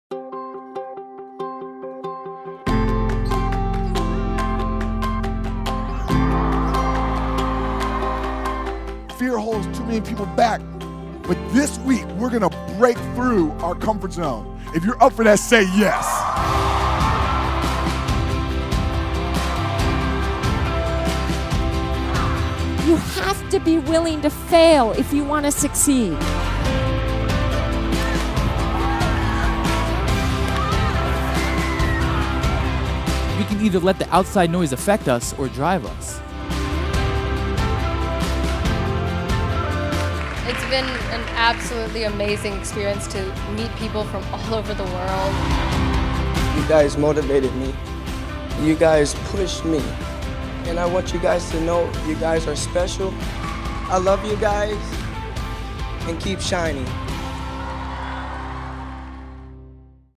Tony Robbins Global Youth Leadership Summit 2019 (1 Min)
Tony Robbins Global Youth Leadership Summit (GYLS) is a four-day program that provides participants, age ranges 14 to 17, with an environment designed to boost leadership skills that will change lives and communities.

GYLS 2019 welcomed over 350 youth from 31 countries.

"We can change our lives. We can do, be, create and give anything we wish." - Tony Robbins